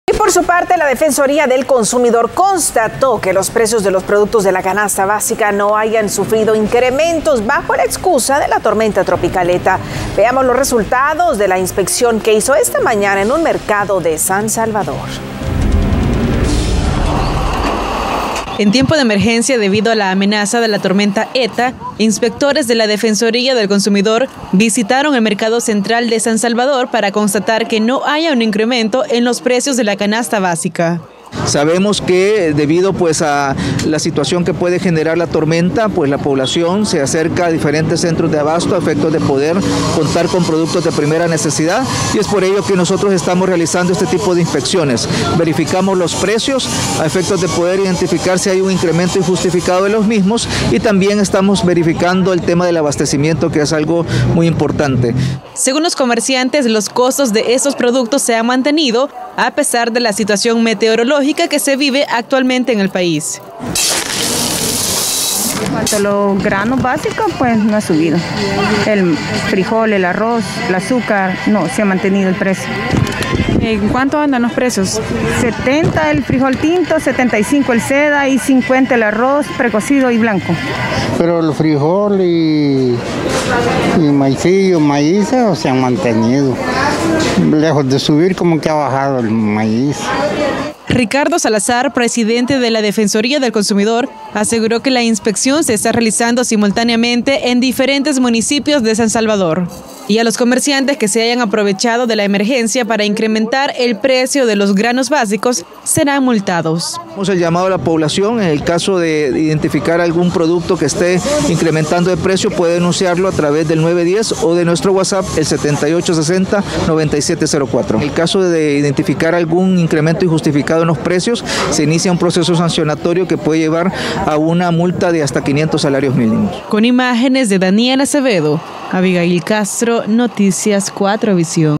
Defensoría del Consumidor verifica precios de canasta básica
La Defensoría del Consumidor constató que los precios de los productos de la canasta básica no hallan sufrido incrementos bajo la excusa de EtaSV. @RaquelCastTCS nos amplia.